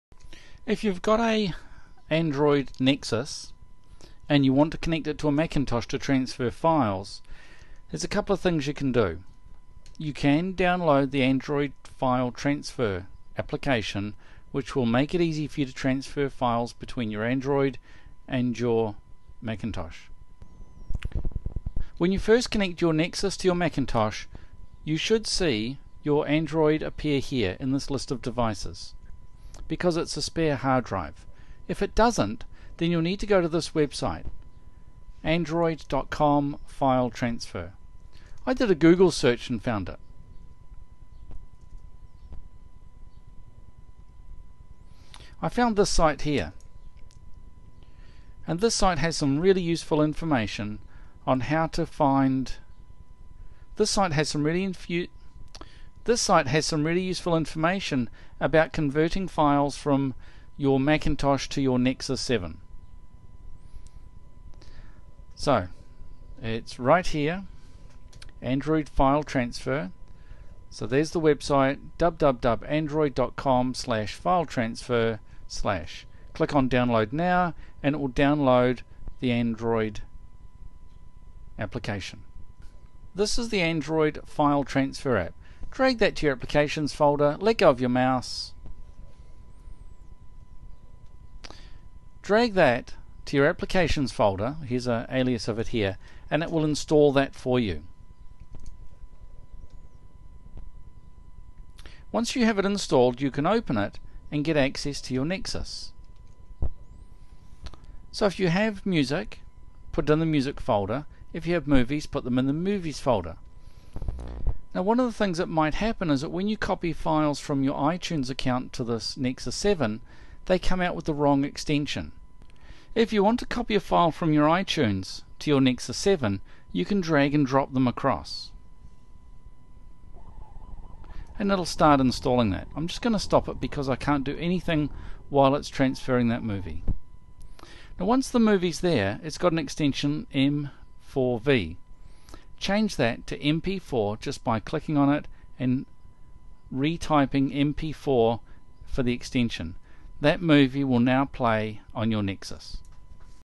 0060 Connecting a Google Nexus 7 to a Mac
You might think that you can connect the Google Nexus 7 directly to your computer. On a Macintosh this often does not work, but there is a solution.

There is an application that you can download from Google that will connect your Nexus 7 to your Mac so you can transfer files quickly. This is not automatic like an iTunes/iPad arrangement, but it is reasonably quick to drag and drop the files into your nexus and have them copy across.